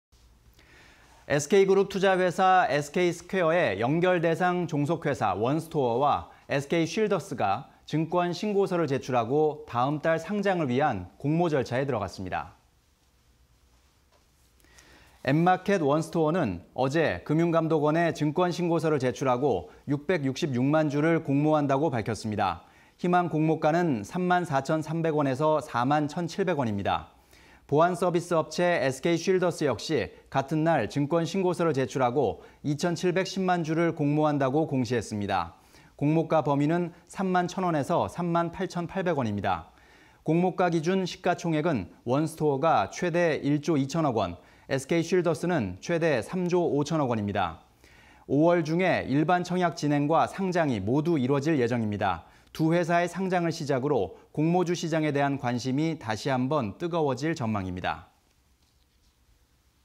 원스토어·SK쉴더스, 증권신고서 제출... 5월 나란히 상장 (20220401)
SK그룹 투자회사 SK스퀘어의 연결대상 종속회사 원스토어와 SK쉴더스가 증권신고서를 제출하고 다음 달 상장을 위한 공모 절차에 들어갔습니다.
앱마켓 원스토어는 지난달 31일 금융감독원에 증권신고서를 제출하고 666만주를 공모한다고 밝혔습니다.
희망 공모가는 3만4300∼4만1700원입니다.
보안서비스 업체 SK쉴더스 역시 같은 날 증권신고서를 제출하고 2710만주를 공모한다고 공시했습니다.
공모가 범위는 3만1000∼3만8800원입니다.
공모가 기준 시가총액은 원스토어가 9000억∼1조2000억원, SK쉴더스는 2조8000억∼3조5000억원입니다.
5월 중에 일반 청약 진행과 상장이 모두 이뤄질 예정입니다.
두 회사의 상장을 시작으로 공모주 시장에 대한 관심이 다시 한번 뜨거워질 전망입니다.